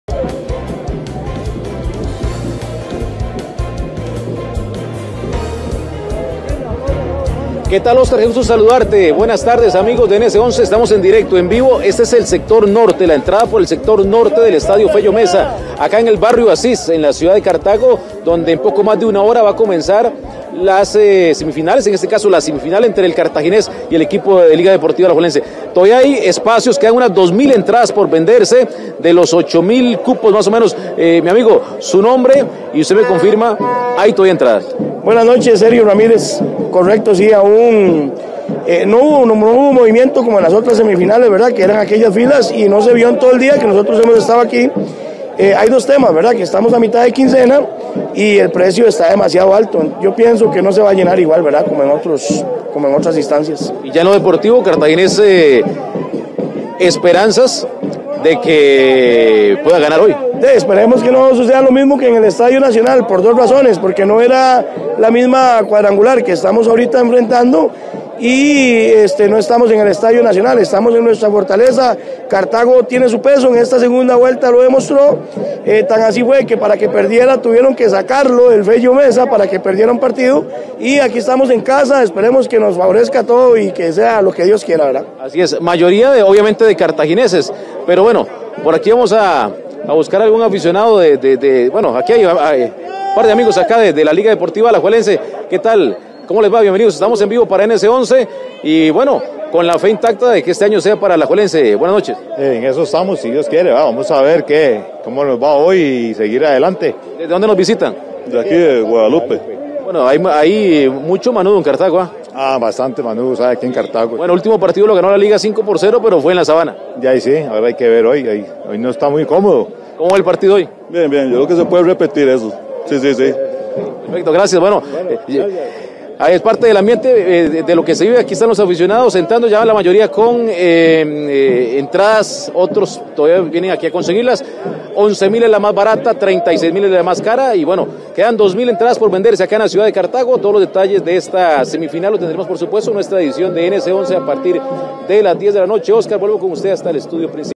Cartaginés y Alajuelense jugarán en pocos minutos la primera semifinal
Los brumosos tienen 14 juegos sin perder en Cartago.
Aún quedan 2 mil entradas por venderse de las 8 mil que se hallaban disponibles.